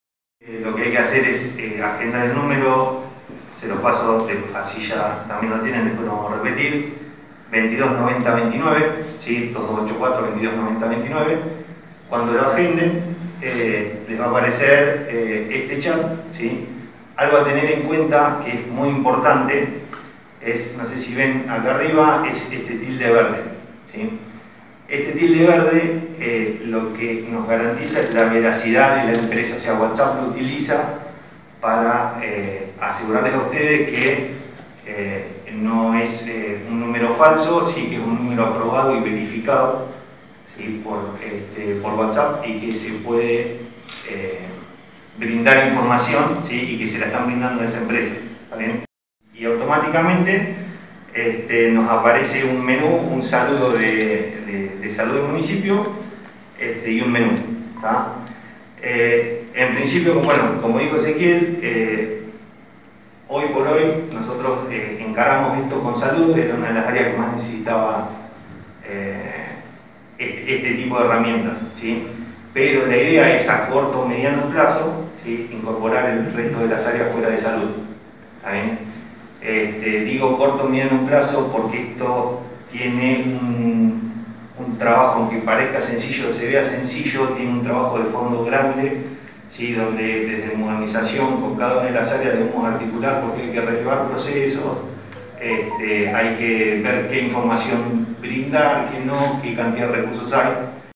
Presentaron el nuevo sistema de contacto con el Hospital
Consiste en un asistente virtual por Whatsapp que será utilizado para responder consultas administrativas. En conferencia de prensa dieron detalles sobre su funcionamiento.